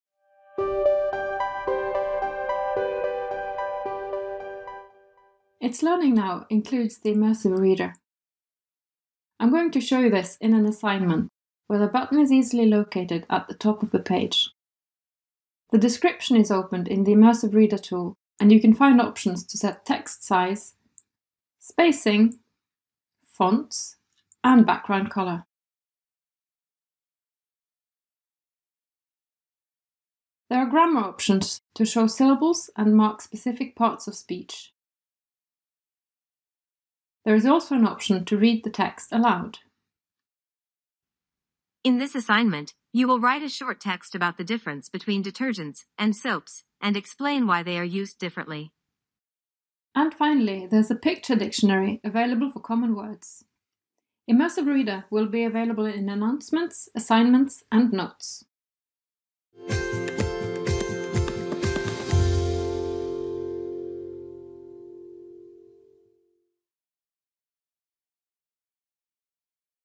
Immersive Reader in itslearning
Immersive Reader is not just another screen reader. It is a literacy tool that can help students with reading or learning difficulties read better. It is also useful for adult learners who are learning a new language, as it includes a picture dictionary for words.